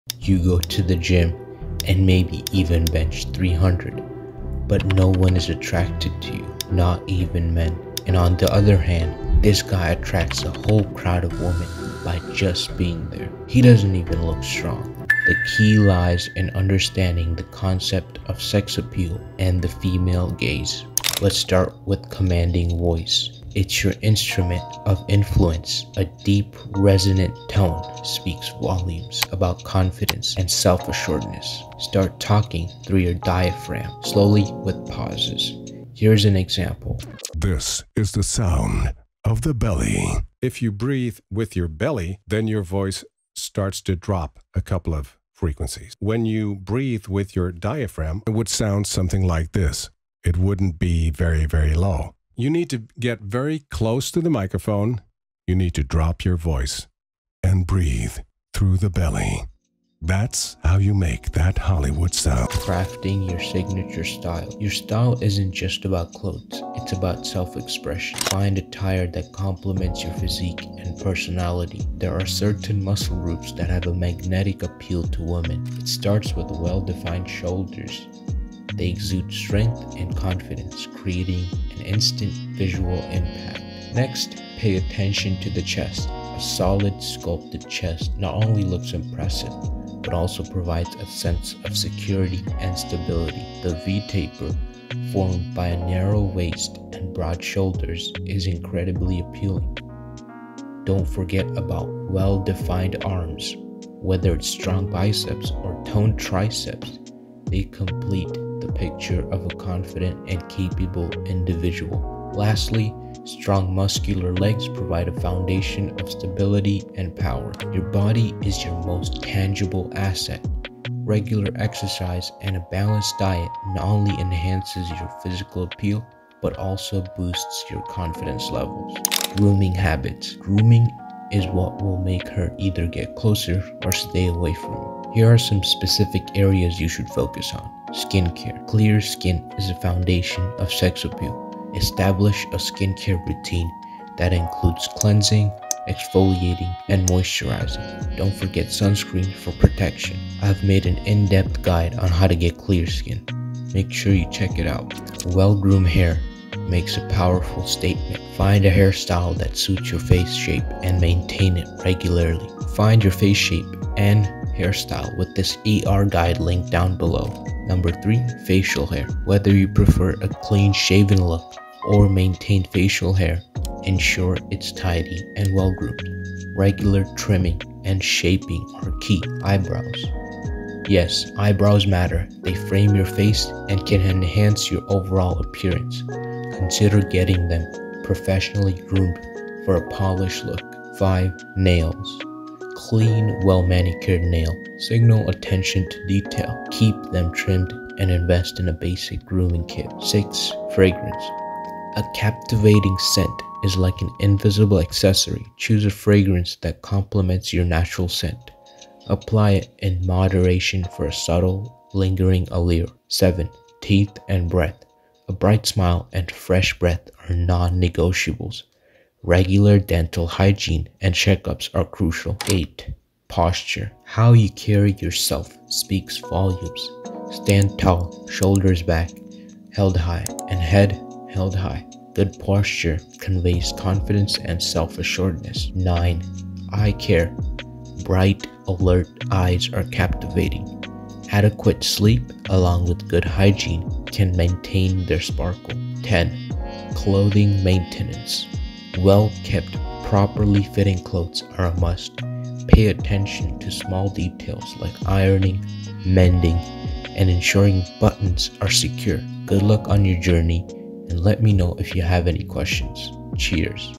women look for this in men (sex appeal)
Discover valuable insights and tips on how to enhance your body and increase your sex appeal. Don't miss out on this opportunity to gain a deeper understanding of the female gaze and improve your overall attractiveness.